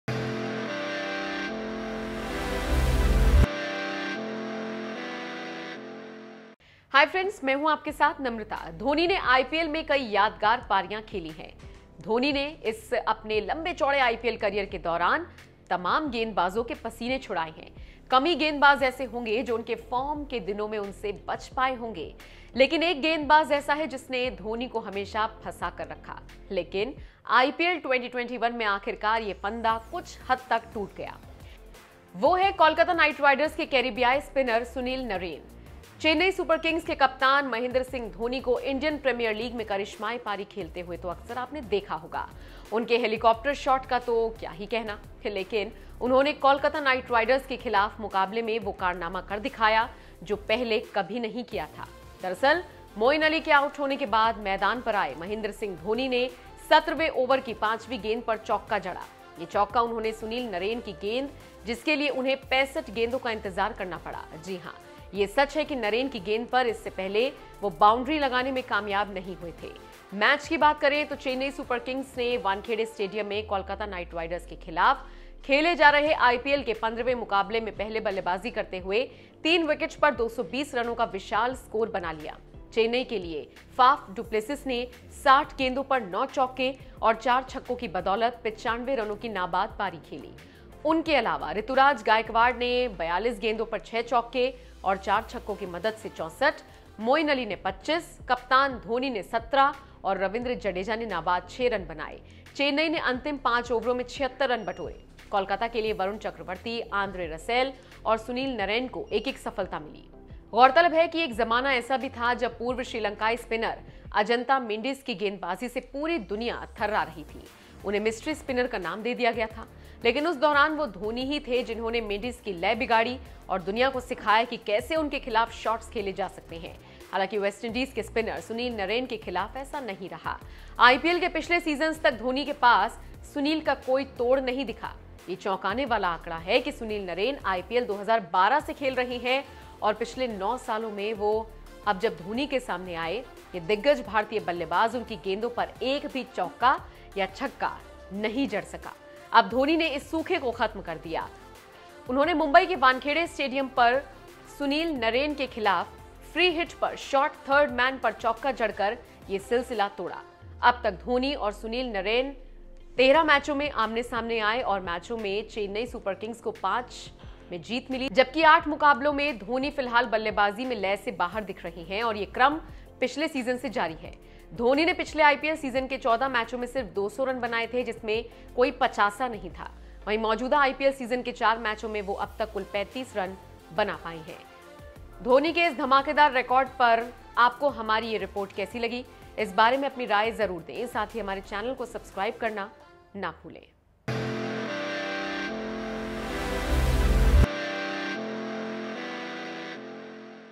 MS Dhoni ने सालों के इंतजार के बाद आखिरकार तोड़ दिया रिकॉर्ड, 65वीं गेंद पर कर दिया कमाल
Dhoni ने IPL में कई यादगार पारियां खेली हैं। धोनी ने इस अपने इस लंबे चौड़े आईपीएल करियर के दौरान तमाम गेंदबाजों के पसीने छुड़ाए हैं। कम ही गेंदबाज ऐसे होंगे जो उनके फॉर्म के दिनों में उनसे बच पाए होंगे। लेकिन एक गेंदबाज ऐसा है जिसने धोनी को हमेशा फंसाकर रखा हैा इस गेंदबाज ने हमेशा धोनी को परेशान किया है।